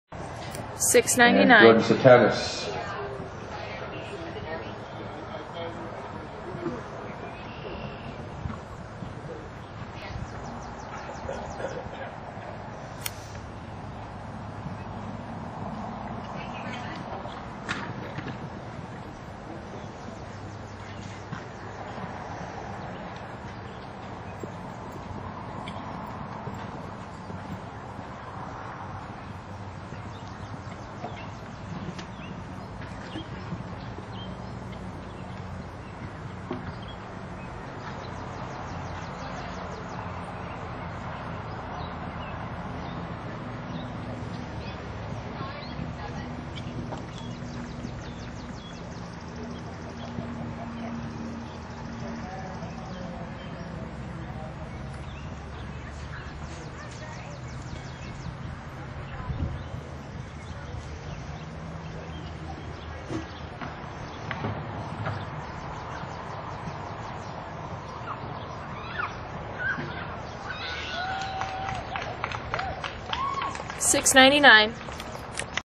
Video of UNDINE ridden by JORDAN SITINAS from ShowNet!
UNDINE ridden by JORDAN SITINAS at Old Salem 2.  This round is from class 407, Low Jr Jpr II2b 1.30 m and finished 15 of 20.